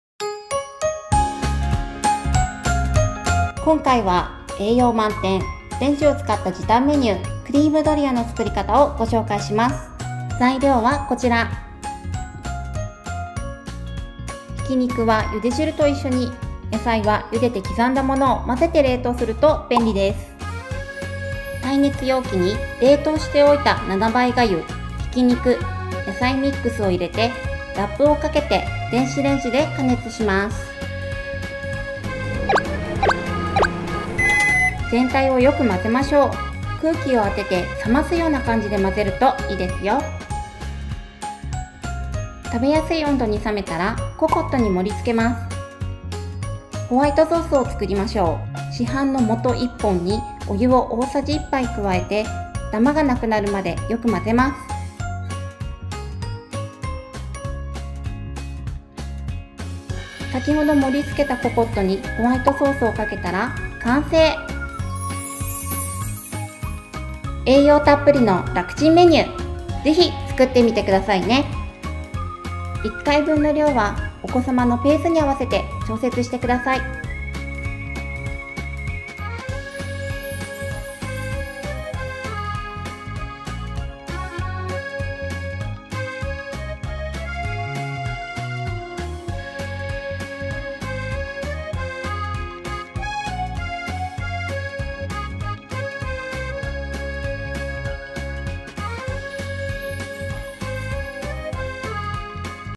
【レンジで時短】冷凍ストック活用クリームドリア【離乳食中期から】
冷凍ストックを活用する、簡単・時短メニュー！　一食で栄養バランスもバッチリ！　離乳食中期からの、クリームドリアの作り方をご紹介します。今回は、和光堂の「手作り応援！ホワイトソース」を使用しています。

【材料】(約1回分)

＊冷凍ストック素材
7倍がゆ…80g
鶏胸ひき肉…10g
野菜ミックス(人参・ほうれん草)…10g

和光堂 手作り応援！ホワイトソース…1本
お湯…大さじ1
※1回分の量は、お子さまのペースに合わせて、調節してください。

イイね👍の評価も大歓迎♫

●関連動画
【離乳食】ダイソー・セリアの100均やSNSで話題の神★商品
【無印良品】離乳食に役立つ便利グッズ３選

この動画の関連情報「ベビー用品準備｜【ベビカム】」